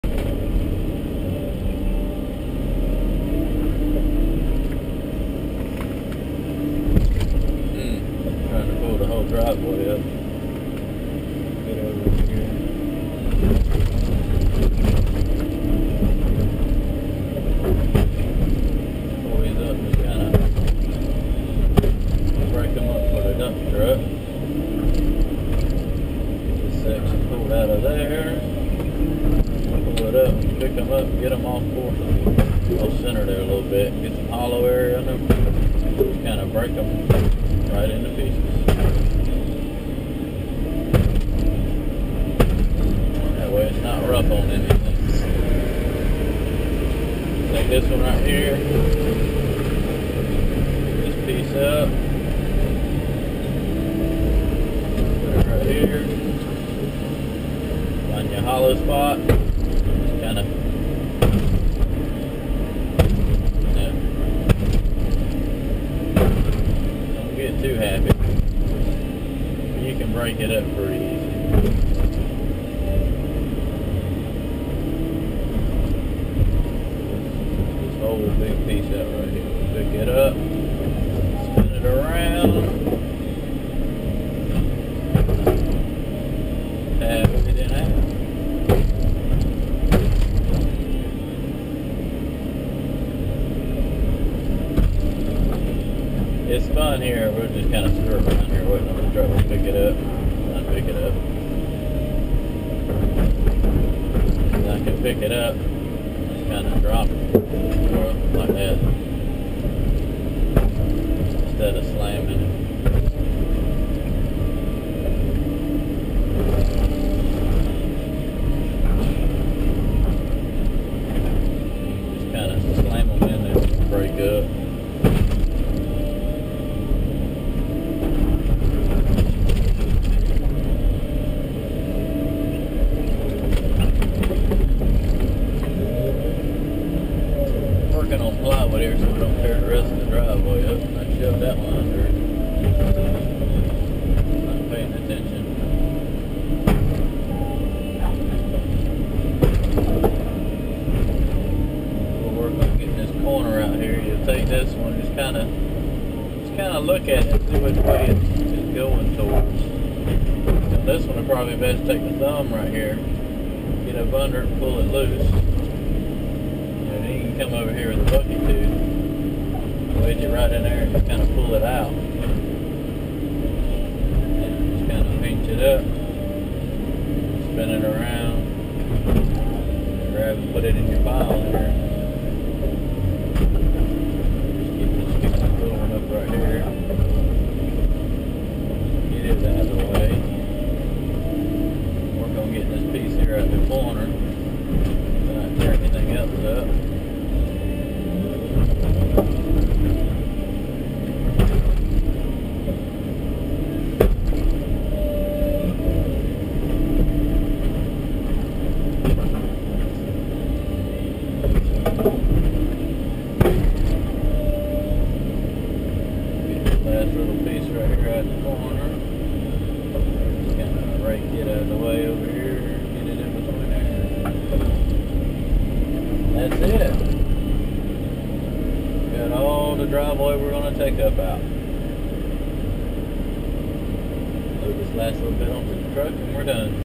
Excavator Busting Concrete
Breaking up the last few sections of a concrete driveway to haul off